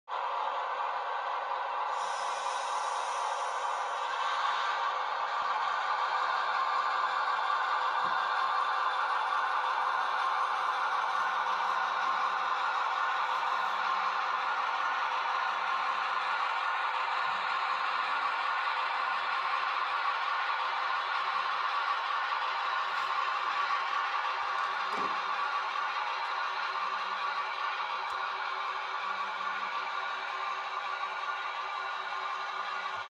When The New Guy Shows Off
Conductor Carl has something to show the boys.